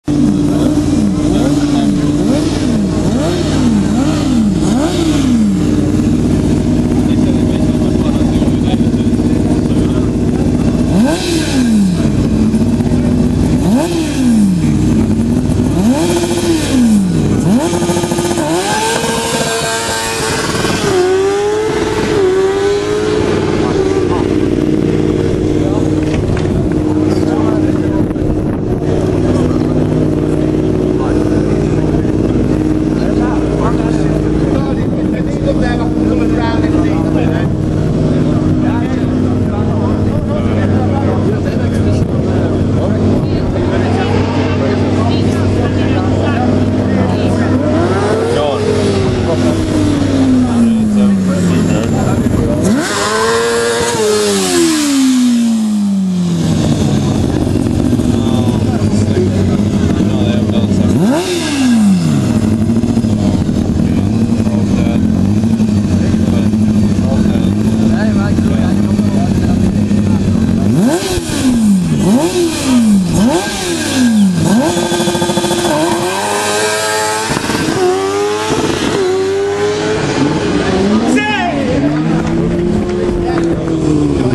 Formula Student 2010
POMOS Reparto Corse acceleration best time, 4.07 s
By Fear!